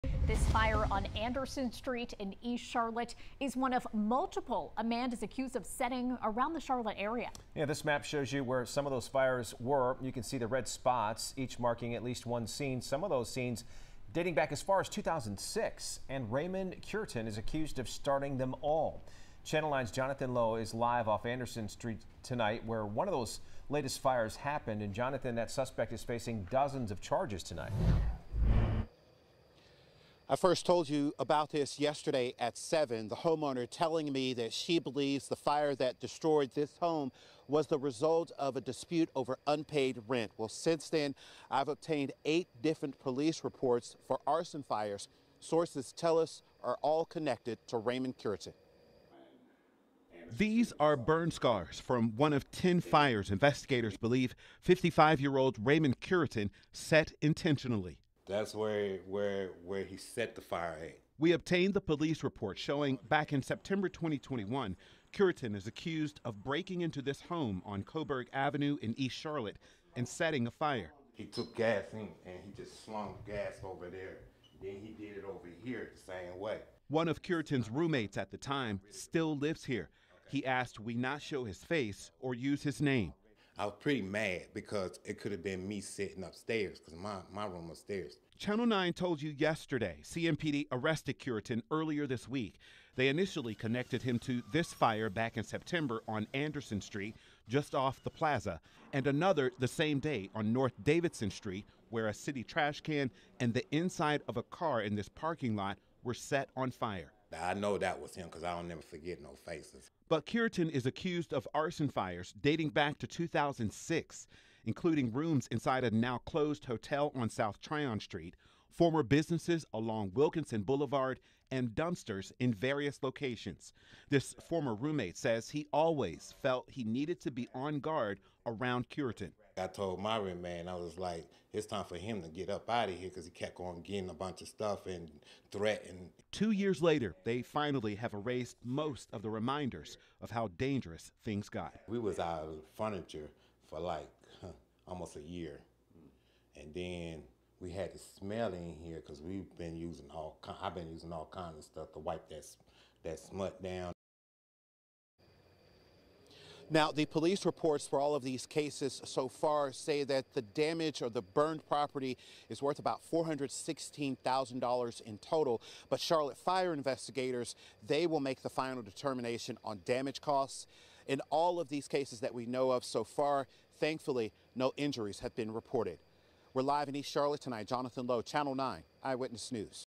Cases involving accused arsonist go back to 2006, CMPD says | WSOC-TV
Raymond Cureton, 55, charged Wednesday with setting a couple of fires is now charged with 10 arsons around the Queen City with some of them dating back to 2006.